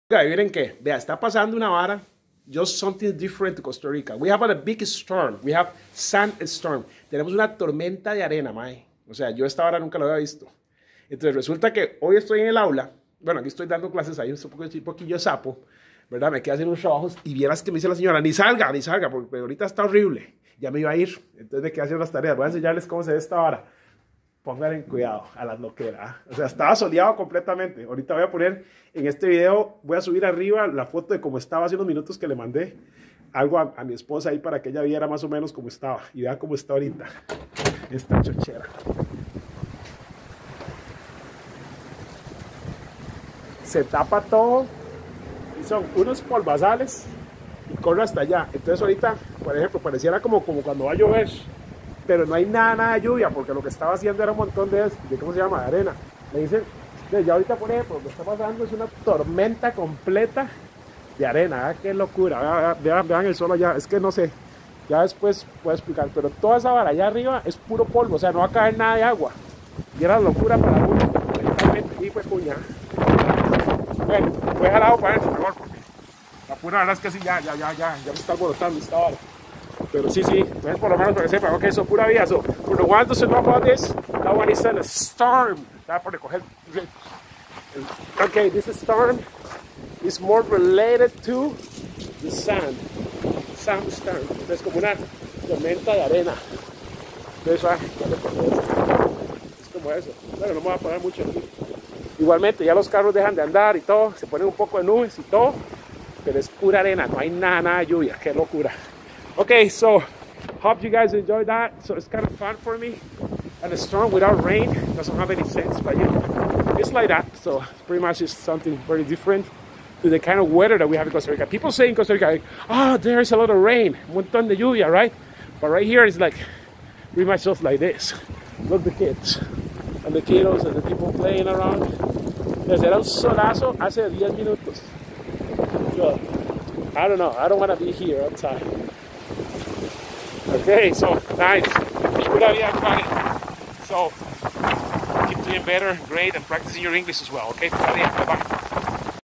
Did you know about Sandstorm in AZ?
Sandstorm in Arizona is somewhat interesting for those of us who live in the tropics since we normally expect rain here that doesn't happen.